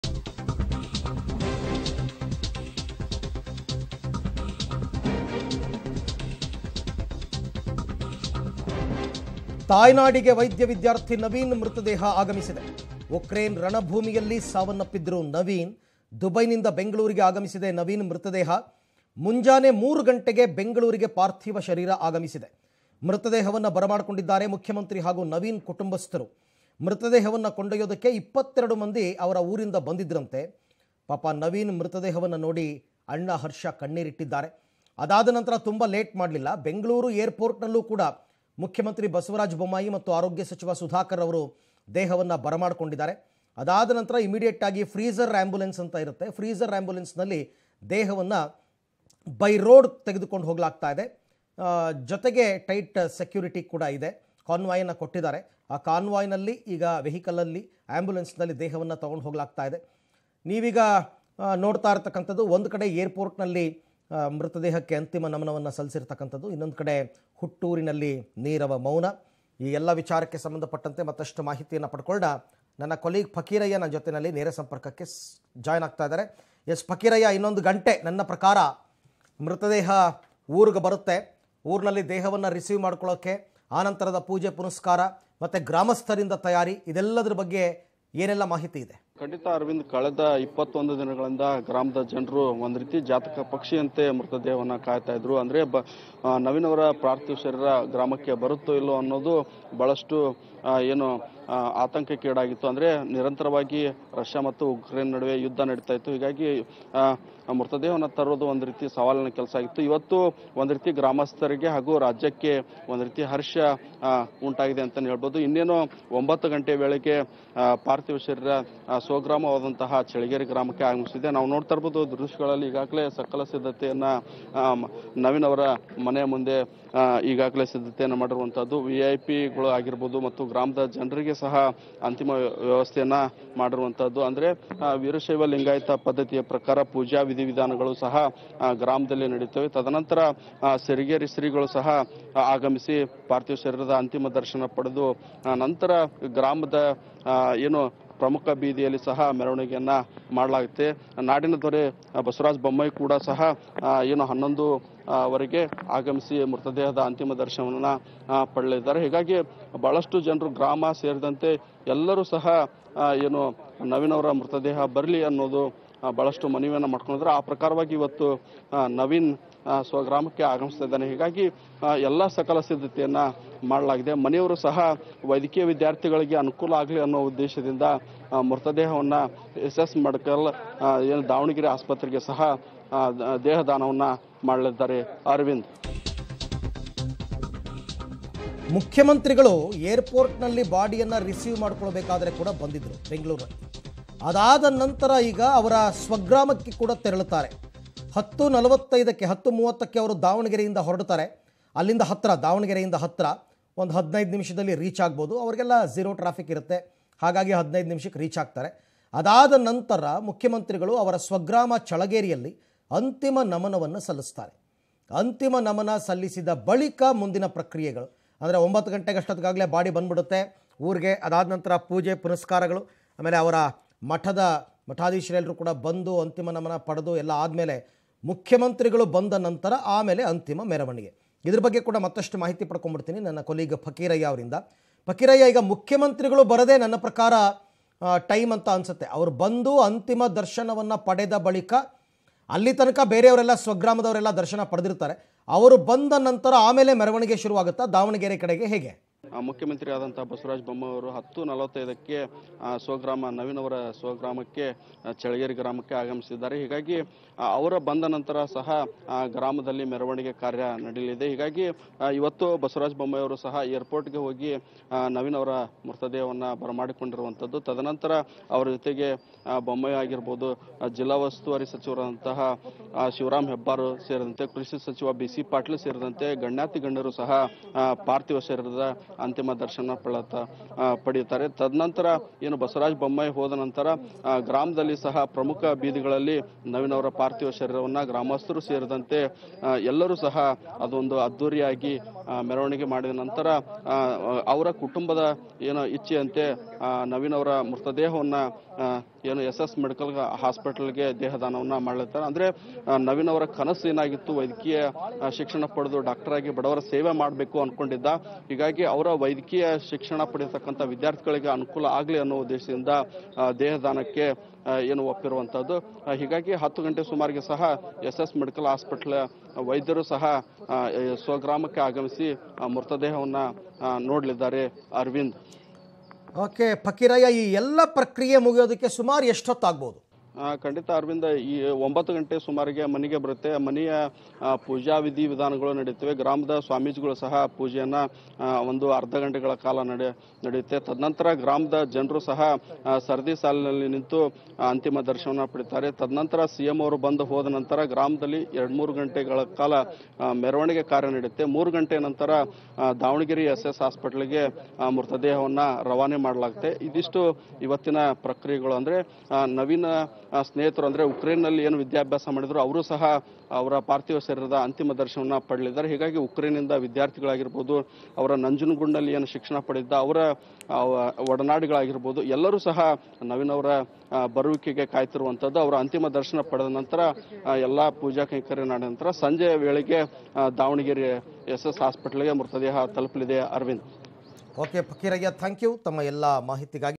Medical Student Naveen Shekharappa's Mortal Remains To Reach His Home Shortly
The mortal remains of Naveen Shekharappa Gyangoudar, an MBBA student who was killed in a shelling attack in war-torn Ukraine️, arrived in Bengaluru in the early hours of Monday. Karnataka chief minister Basavaraj Bommai among others paid his last respects to the student. He was killed on March 1 in Kharkiv city of Ukraine.